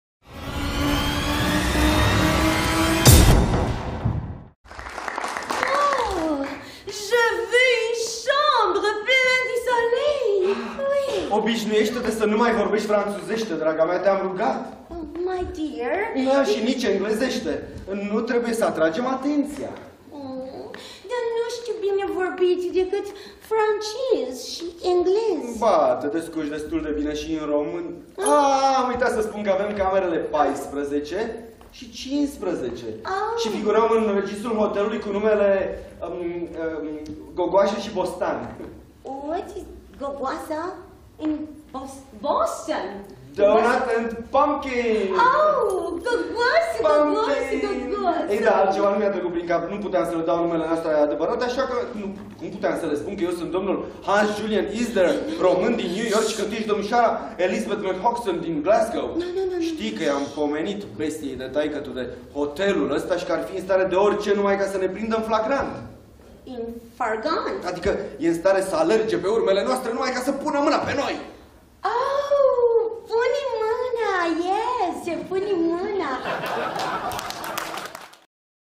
Comedia ”Interpretul” la Teatru la Cinema
Pentru voi a doua ‪#‎pastiladecomedie‬!
Vino să vezi un spectacol unic, ”Interpretul”,Sâmbătă, 30 Iulie, ora 20:00 la Teatru la Cinema din Cinema Muzeul Țăranului!

Oare, COMUNICAREA este cheia fericirii noastre ?
Oare, va găsi interpretul, soluția de a ieși din toate încurcăturile în care singur s-a băgat ?
Oare, vorbind mai multe limbi poți stabili relații mai apropiate și mai oneste cu mai mulți oameni ?
La aceste întrebări, doar dumneavoastră dragi spectatori, veți putea răspunde vizionând  comedia muzicală INTERPRETUL!

Te așteptăm lateatru!